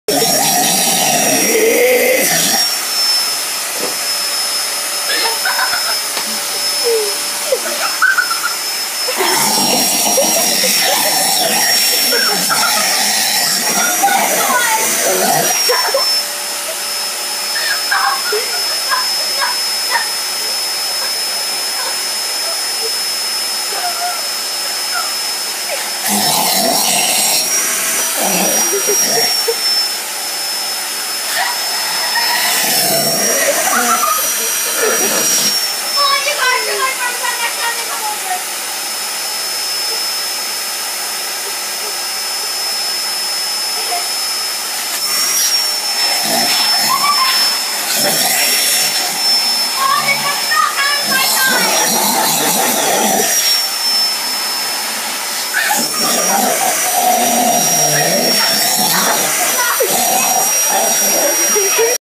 My Dog Jasper Eats the Vacuum
My funny dog Jasper tried to eat the vacuum cleaner whenever we
try and use it around the house. He seems to like it. He sure is